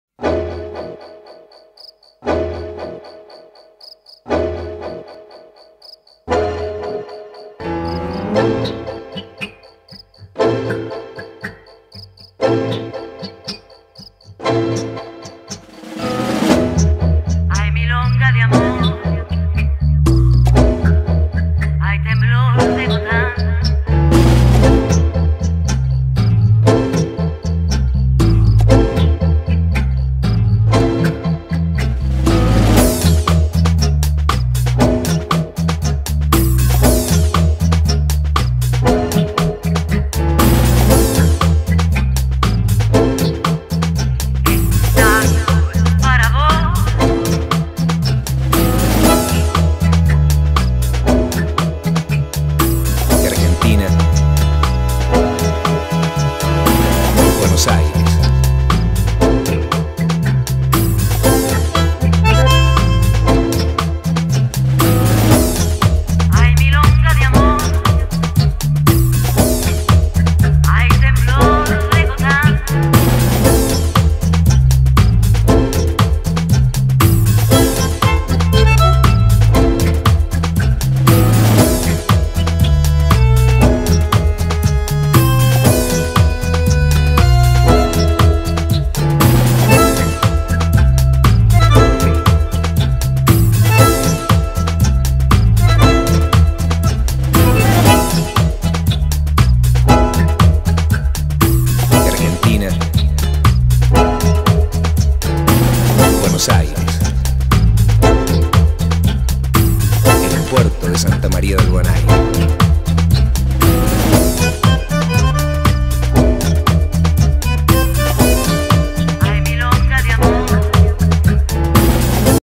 Gotan Project | Las Pegadas 2021 | Salsa Bachata
Gotan Project was our inspiration to create a dance piece with all our semi-profi female dancers during our dance sport camp in Wieleń Zaobrzański in June 2021.